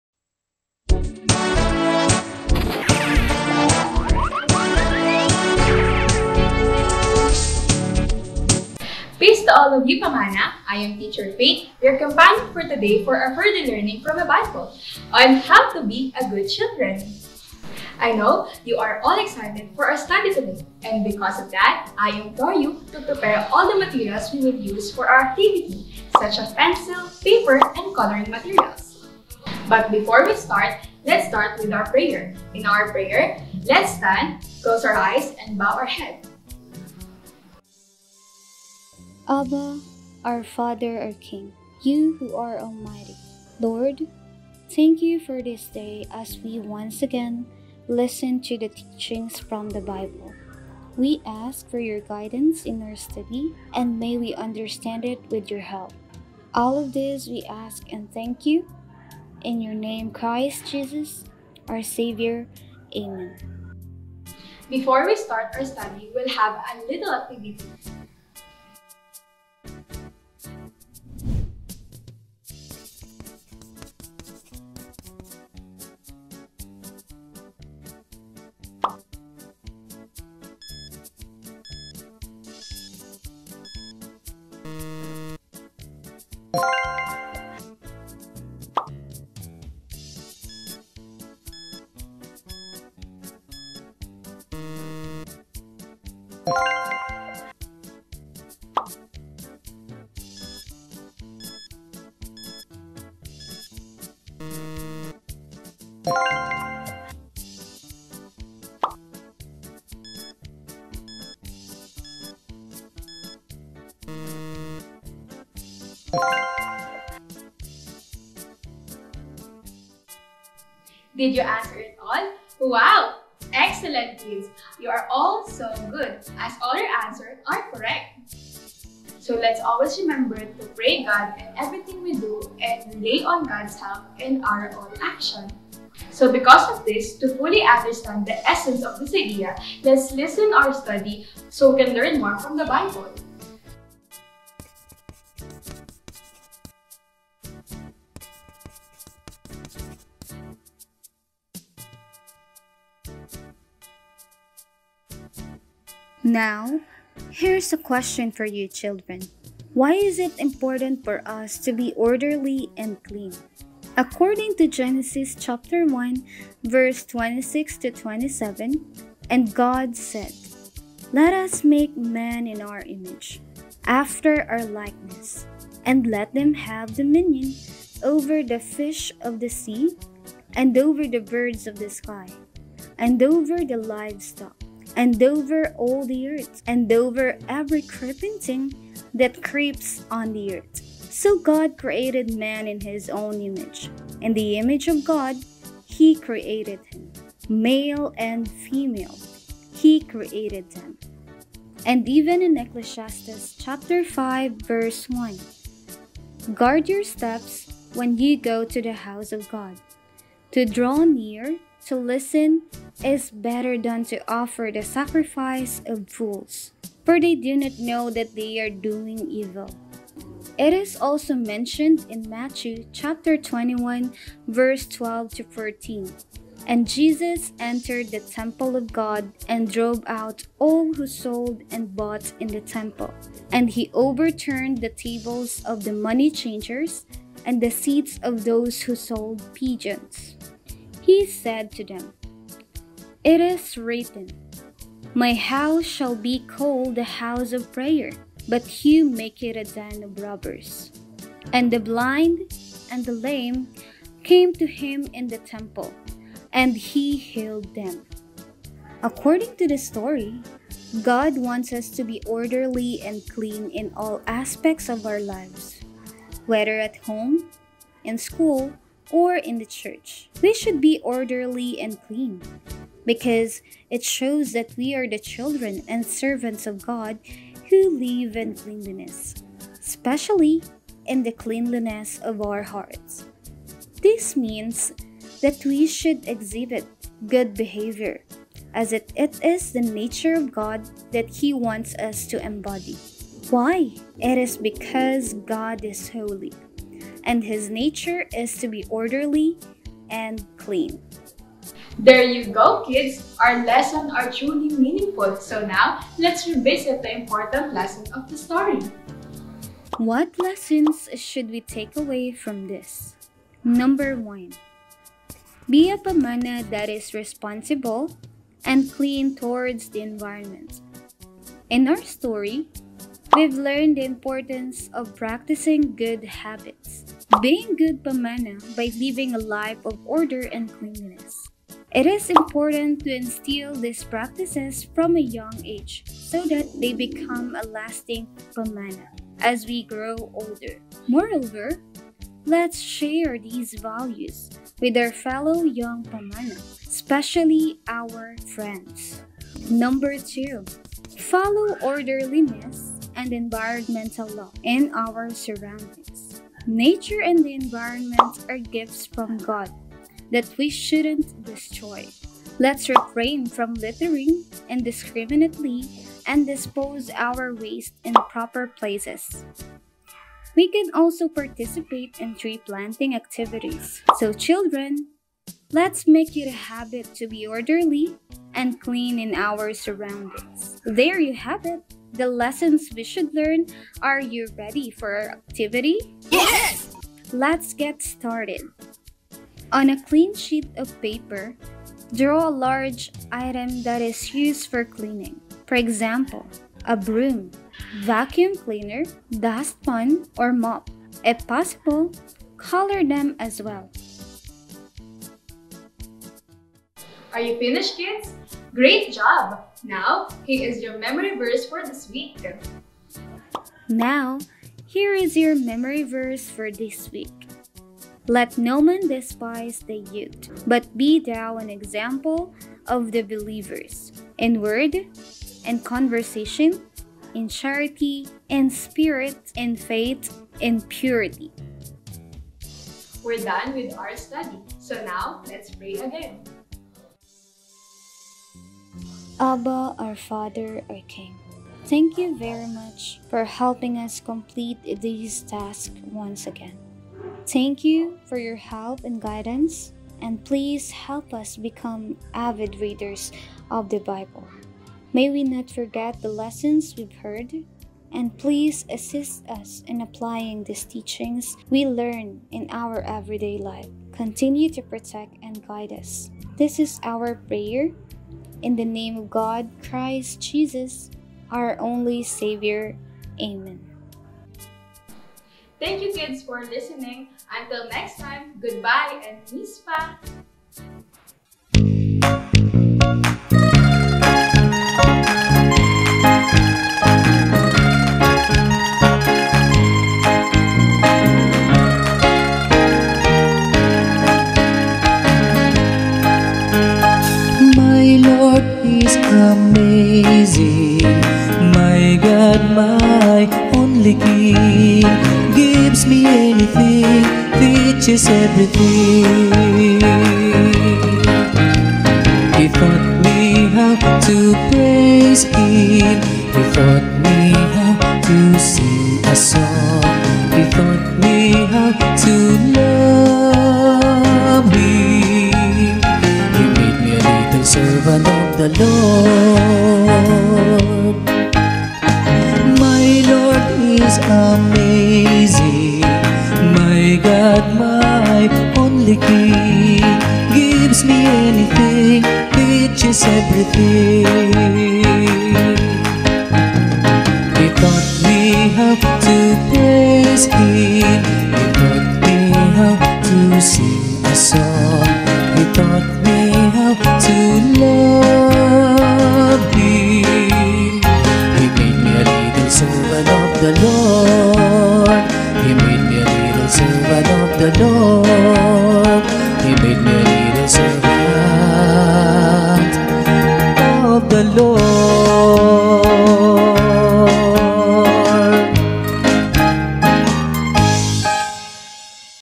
The Pamana Who is Caring Towards Their Environment | English Bible Story
Shalom! Here is Our English Version of Pamana video. Let us Guide and support our beloved Pamana on their Online Worship Service. Thank you.